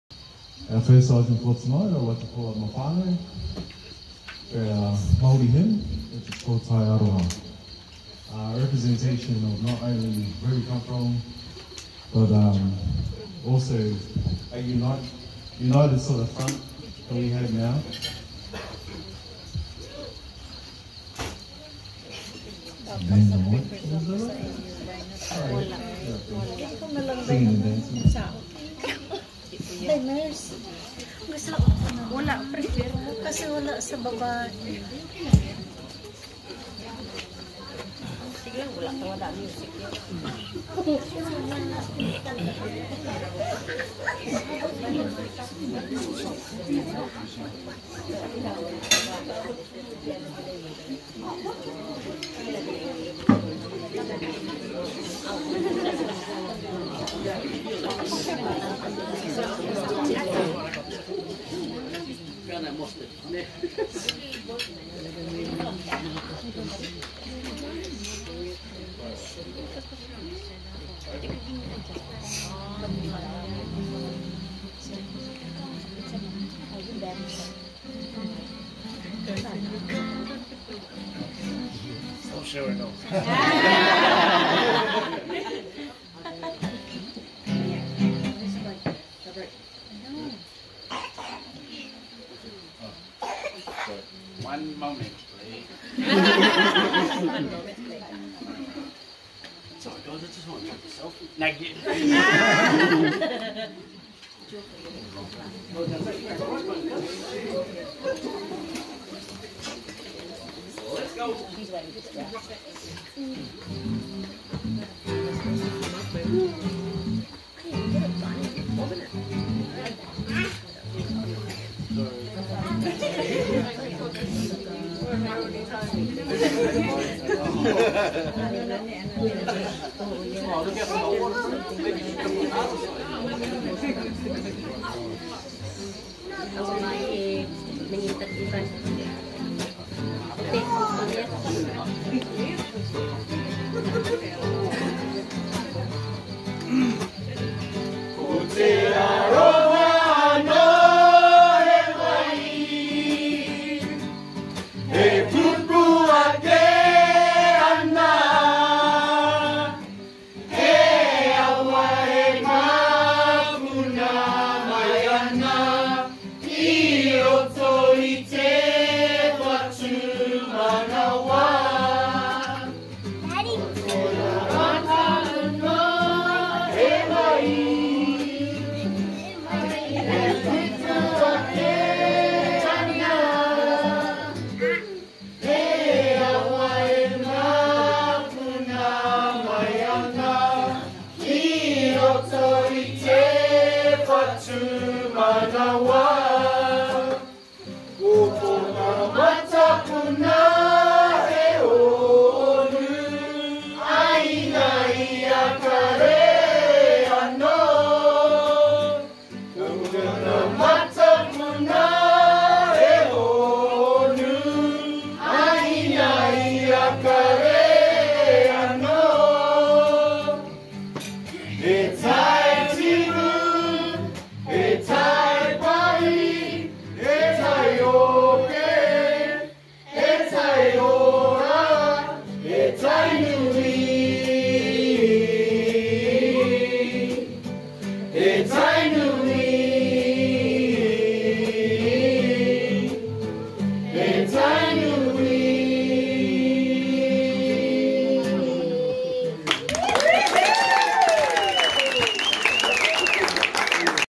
Beautiful New Zealand Cultural Wedding Song
Groom's Family serenading the Bride and Groom.